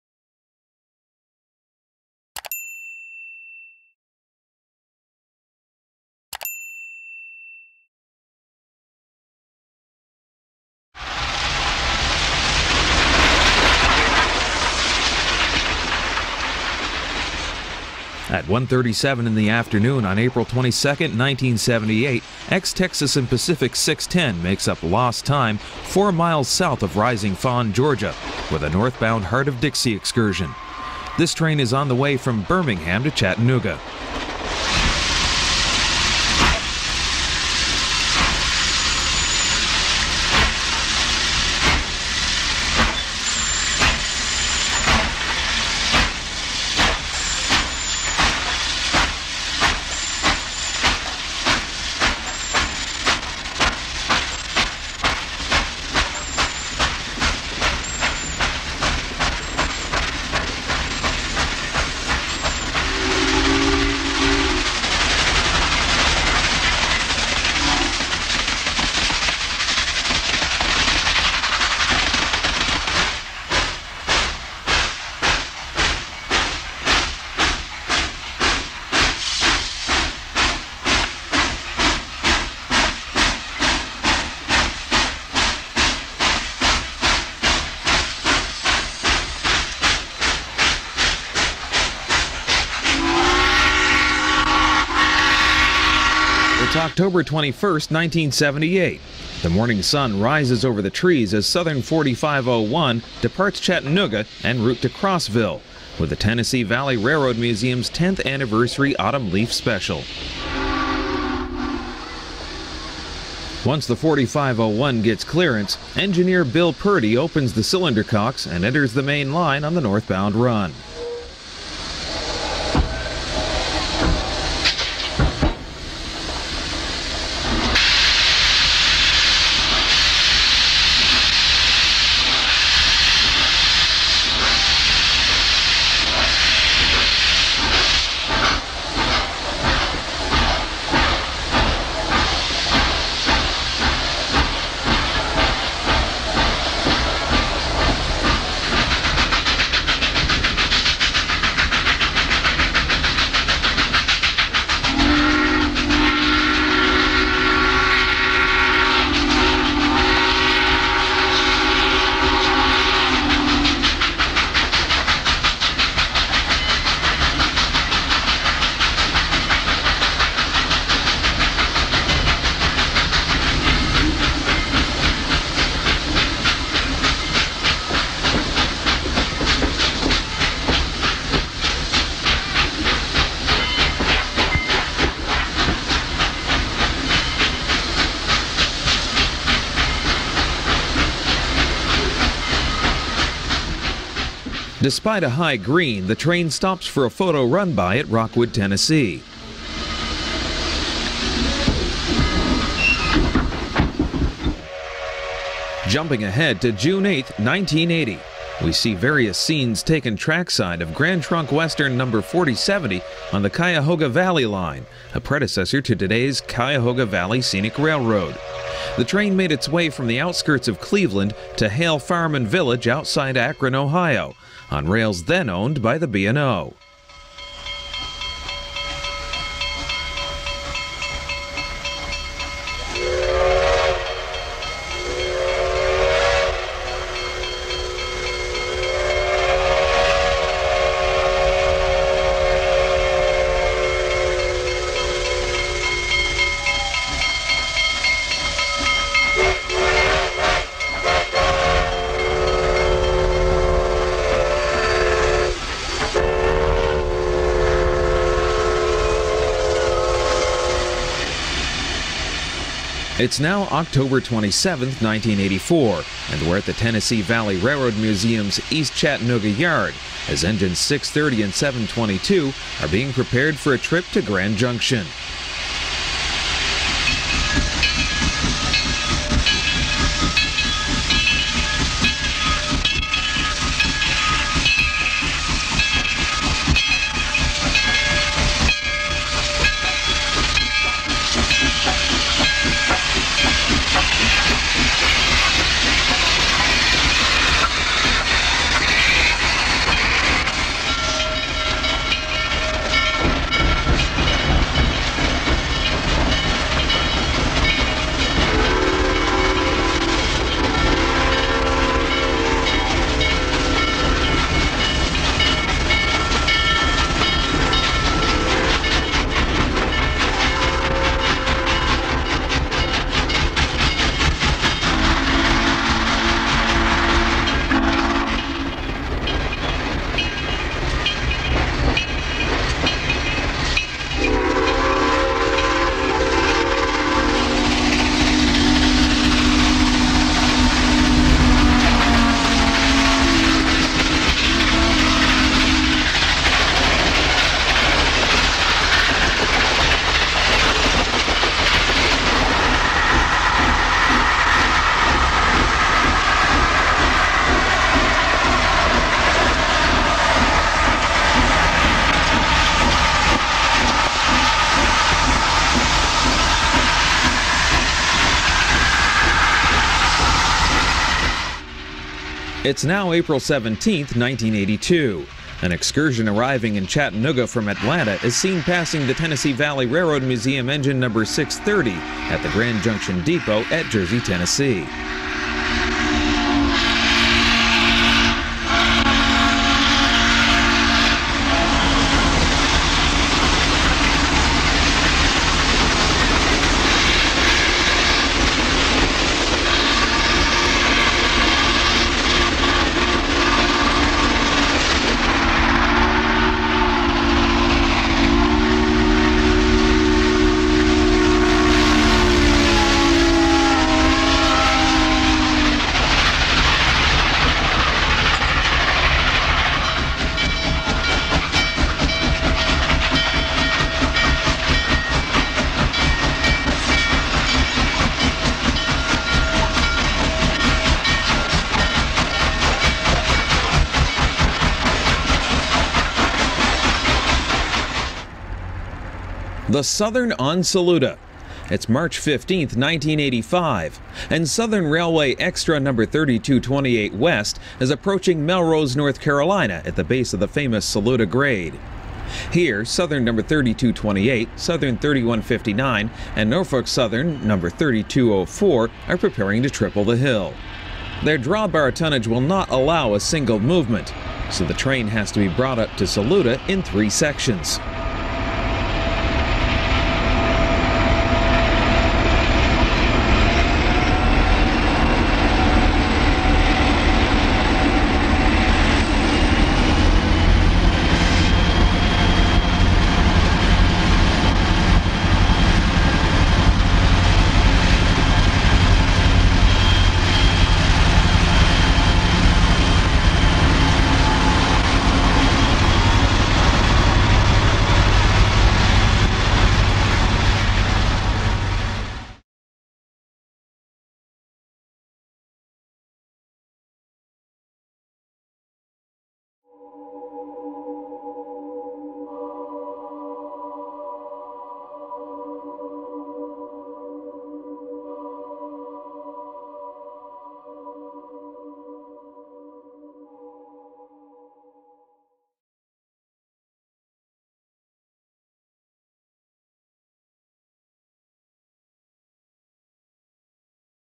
Classic 1970's & 80's Steam Train Runs with original sound
--Super 8mm film with original Magnetic Striped Audio--
Join Green Frog Productions for a nostalgic look back during the zenith of the Southern Railway steam program in the late 1970's and early 80's in and around Chattanooga Tennessee.

Follow Filmographer, Tom Rock as he records Texas & Pacific No. 610, Chesapeake & Ohio No. 2716 and Southern Railway No. 4501 making their way in and out of Chattanooga from points north and south.

Also included is a short segment on the Cuyahoga Valley Line from Cleveland to Akron Ohio, featuring Grand Trunk Western No. 4070.

Loco's Included:
#610, #2716, #4501, #4070, #630, #611, & early Diesel action

Time Approx: 82 Minutes